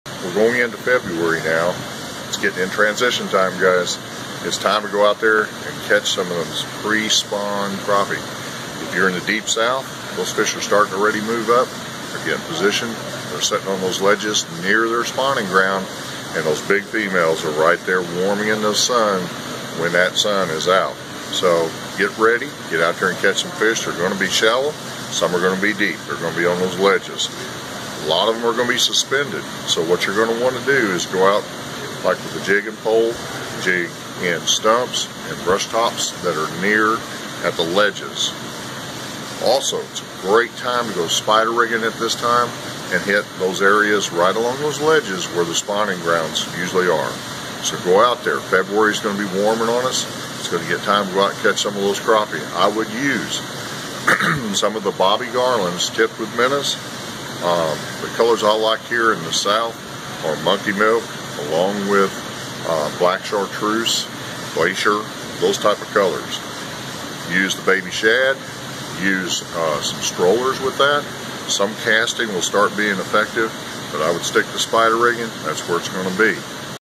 Fishing pre-spawn crappie? Try these tips from Crappie Dan!
Fishing pre-spawn crappie this month? Check out some of these tips from @crappie.dan! Be sure to check out the full video for more information on why vertical jigging and spider rigging with Bobby Garland Crappie Baits and @bnm_poles can be ideal for crappie in the pre-spawn.